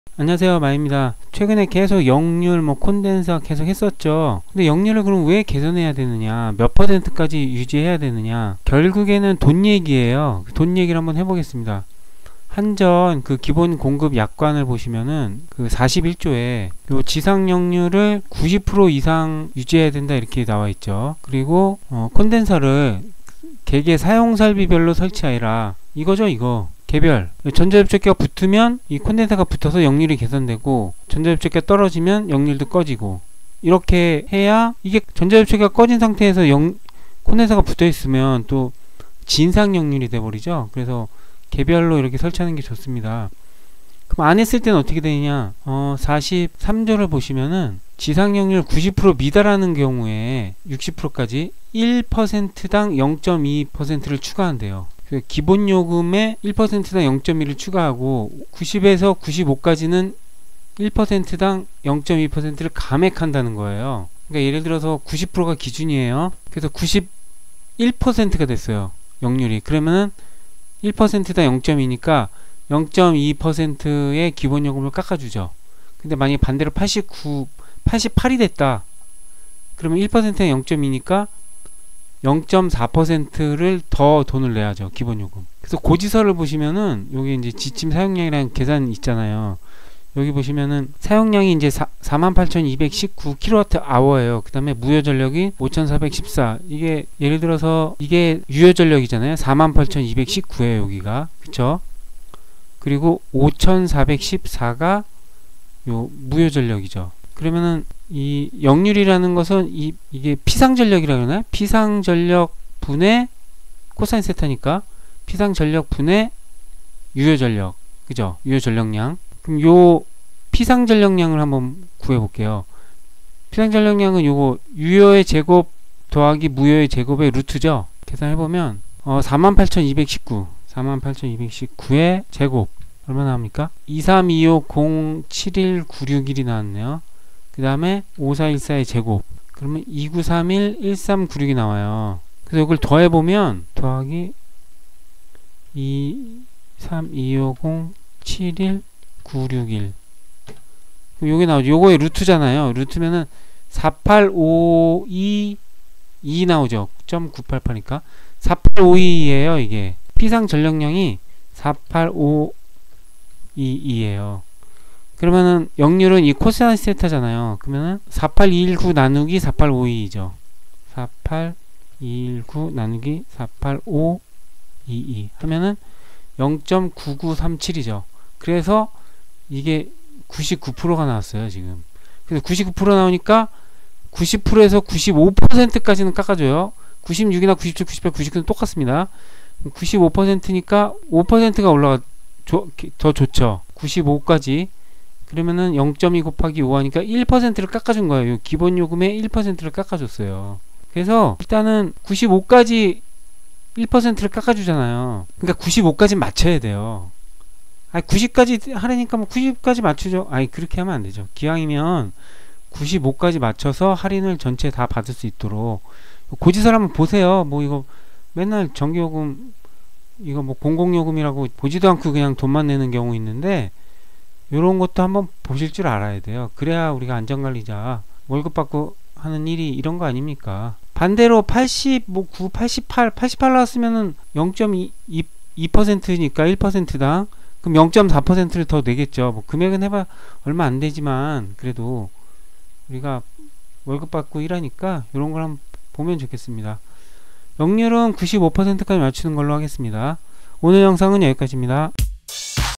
[222]역률을 개선해야 하는 이유(돈얘기)
전기요금 절약을 위한 역률개선에 대해 알아 봅니다.